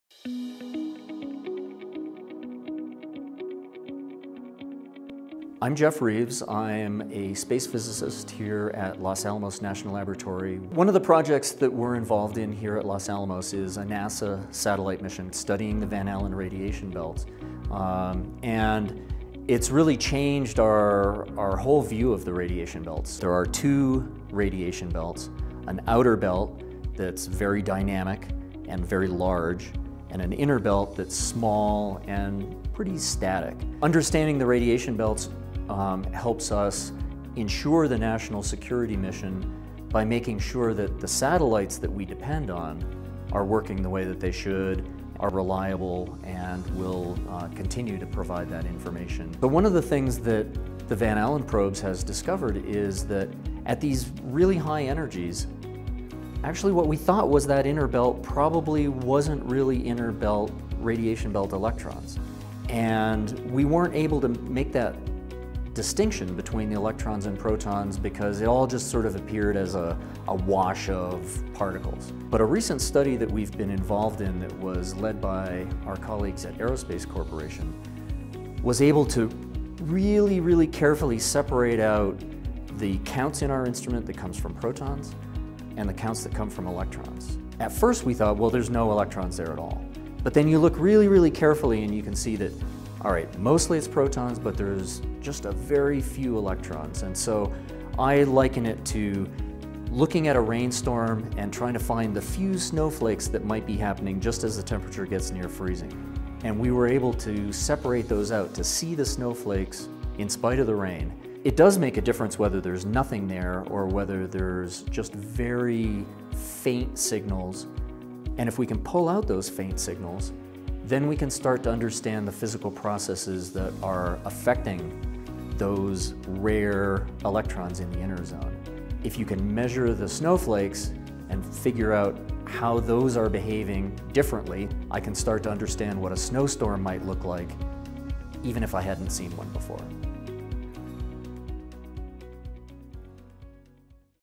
Electrons in the Inner Van Allen Belt
A recent study shows that the inner Van Allen belt has less radiation than previously believed. Observations from NASA’s Van Allen probes show the fastest, most energetic electrons in the inner radiation belt are actually much rarer and harder to find than scientists expected. This is good news for spacecraft that are orbiting in the region and can be damaged by high levels of radiation. The results will also help scientists better understand—and detect—effects from high-altitude nuclear explosions.